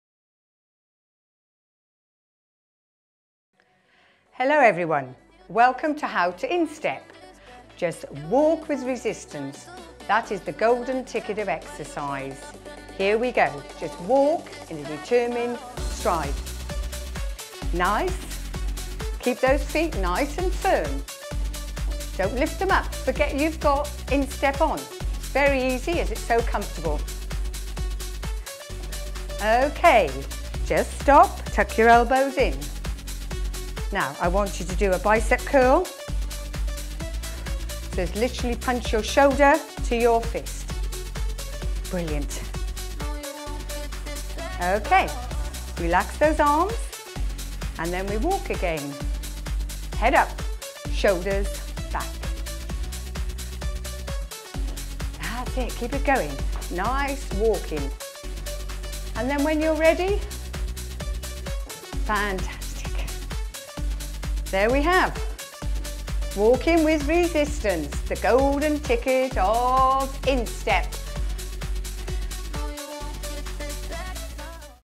How to Instepp
Now you've got your Instepp, here's how to use it!
Instepp inventor Julie Ford shows you how to use Instepp correctly.
Instepp is a fitness tool that tones and conditions your whole body through functional movement - walking or sitting, plus resistance!